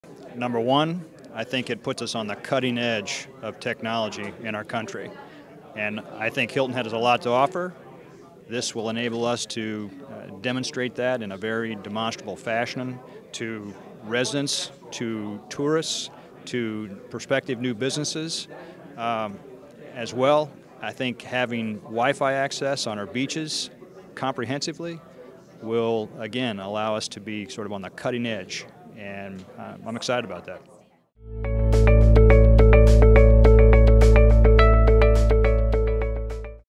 David Bennett on Lowcountry Broadband Plan
Town of Hilton Head Island Mayor David Bennett shares thoughts on the Lowcountry Broadband Plan